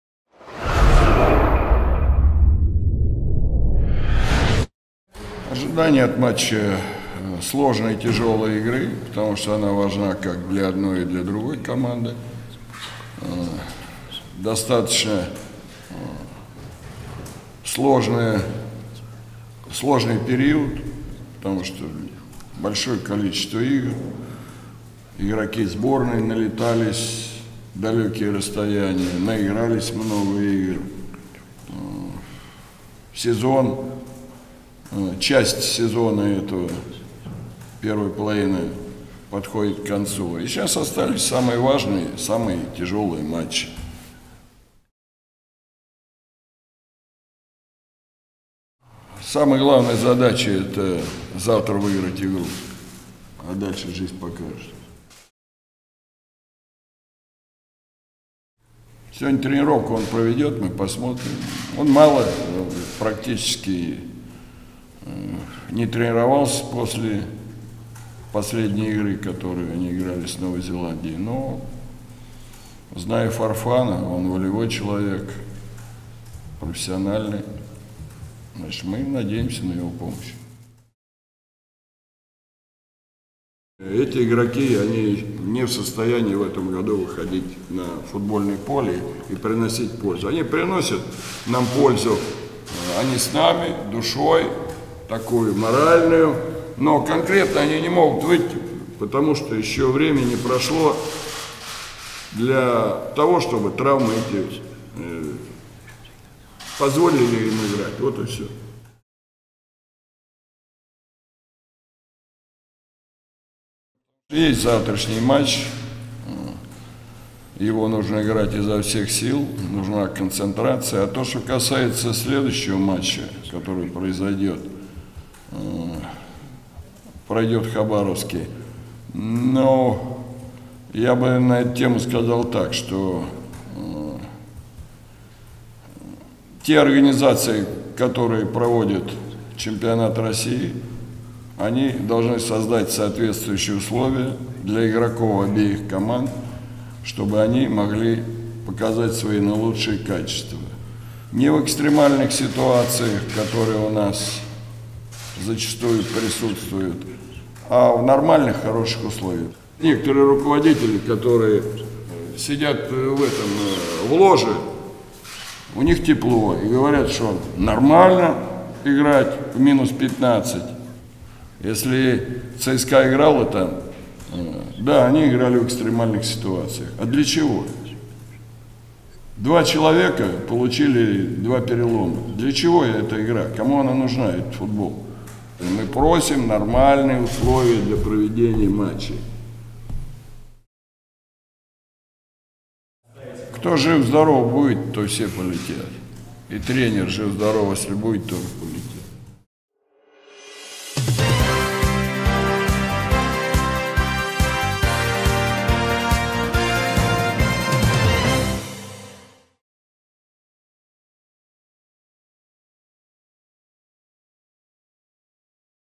Юрий Сёмин: В матче с «Копенгагеном» нужен характер
Пресс-конференция Юрия Сёмина перед игрой с «Копенгагеном» в пятом туре группового этапа Лиги Европы.